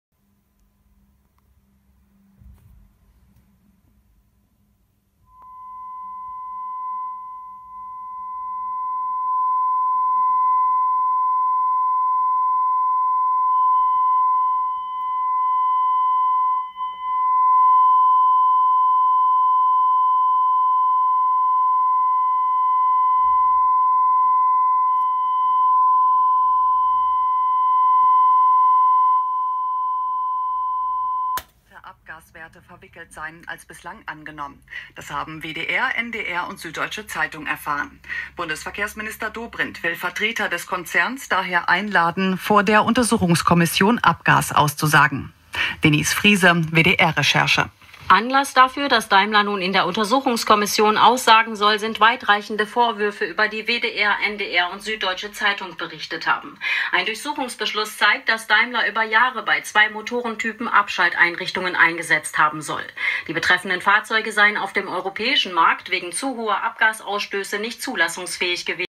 SIEMENS 8" field coil speaker test PART I
Here i am showing a short test of a vintage german fieldcoil speaker, made in 1938 by SIEMENS.
I used 1kHz tone generator and DC power supply .
The maximum DC voltage that was used for the speaker was 80 Volt , thats was the maximum power for these speaker